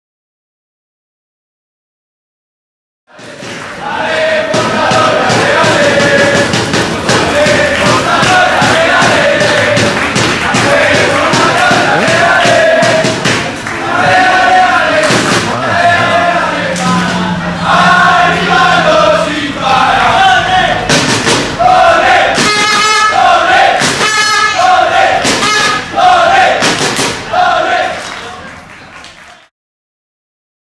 FrenteLodón
Vídeo del FrenteLodón animando al Torrelodones C.F. Femenino en la jornada 5 de Segunda División Femenina contra el Rayo Vallecano "B".  11 de octubre de 2015. Campo: Julián Ariza de Torrelodones